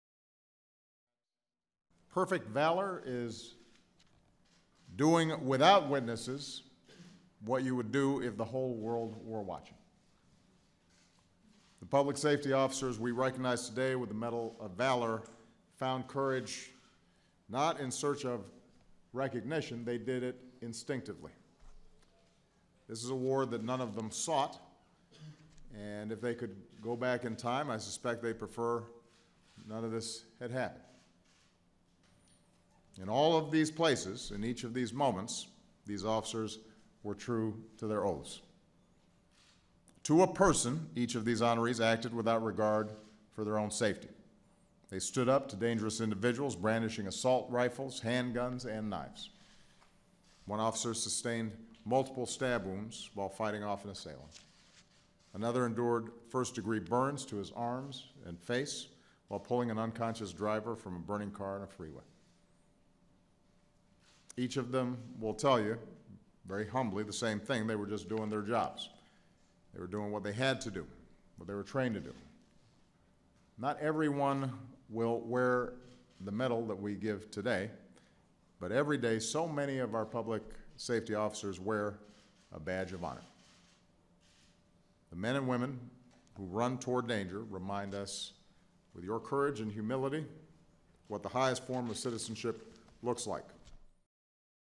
Obama Awards Medal of Valor to Law-Enforcement Officers
President Barack Obama on Monday awarded 13 law-enforcement officers the Medal of Valor, the nation's highest honor for officers who put themselves in harm's way. Photo: AP